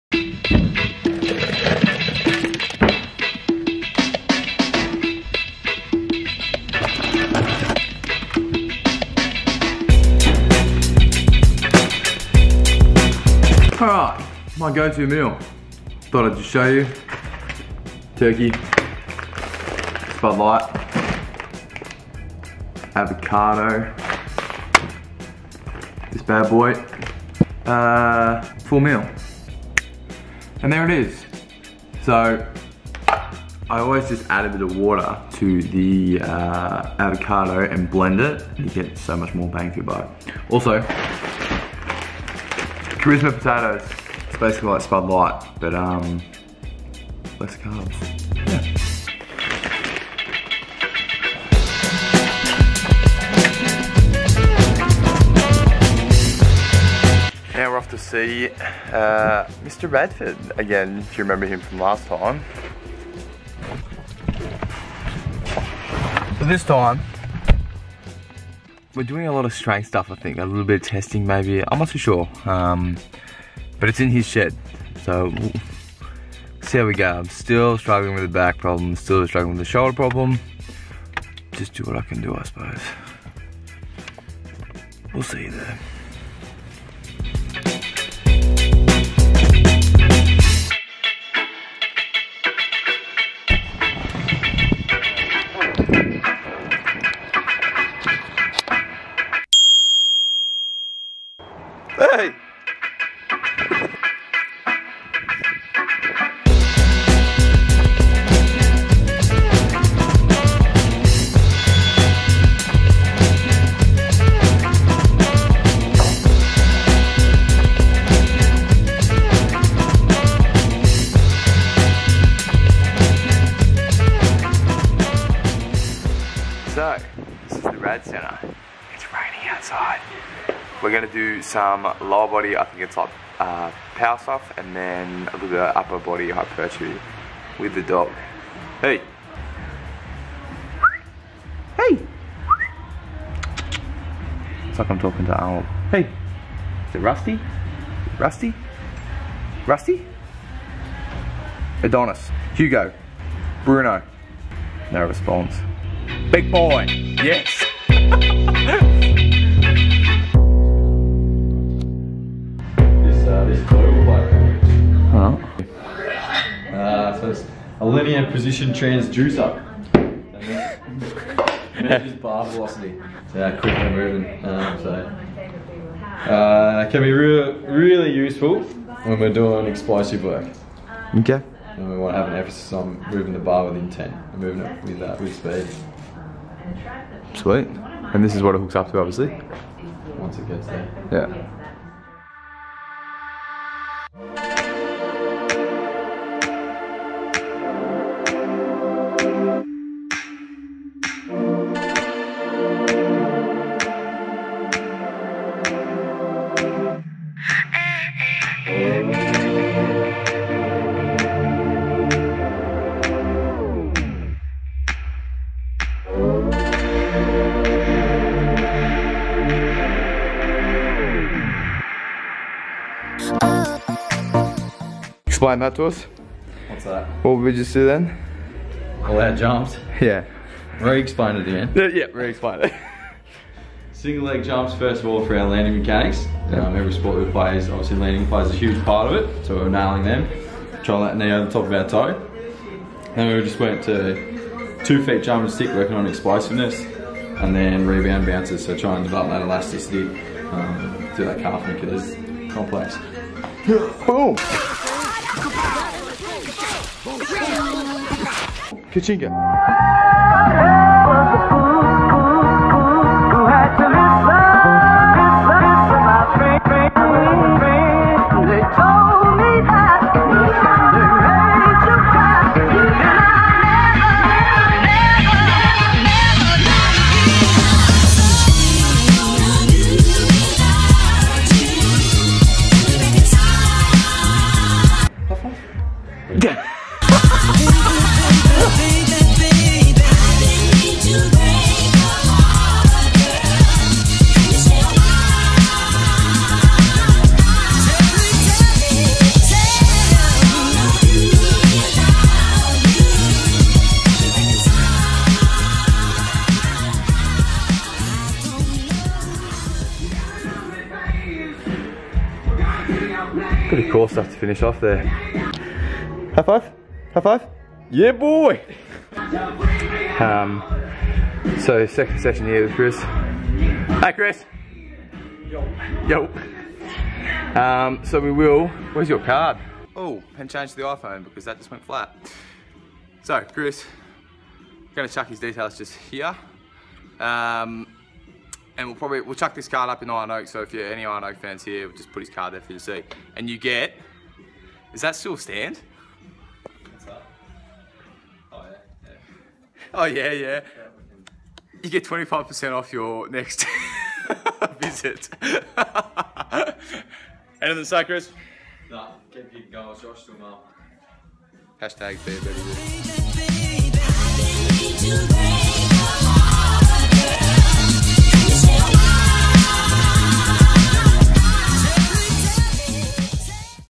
Rad Centre Athletics
In today's vlog I share with you an easy recipe that is one of my go-to meals when I need something quick, easy, and filling that fits well within my macros.

I also caught up with Chris Radford from the
RAD Centre again (radcentre.com.au) for a lower body power and upper body hypertrophy session, with a little bit of core work thrown in for good measure. It's great to work with Chris as he breaks down even seemingly basic exercises to give a clear understanding of the benefits.